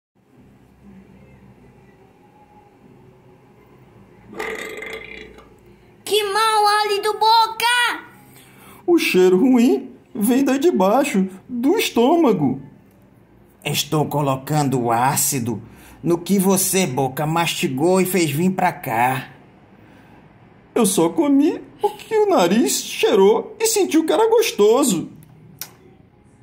Assumo os meus erros ou transfiro para outra pessoa? 👃 👄👈 (português)
Agradeço ao meu filho David por ter me ajudado neste vídeo.

O nariz reclamou com a boca:
“Que mau hálito, boca!”.
A boca, por sua vez, queixou-se ao estômago:
“Que cheiro ruim vem do estômago!”.
O estômago protestou para a boca:
“Estou colocando ácido no que você, boca, mastigou e fez vir para cá!”.
A boca, assim, objetou ao nariz:
“Eu só comi o que o nariz cheirou e sentiu que era gostoso!”.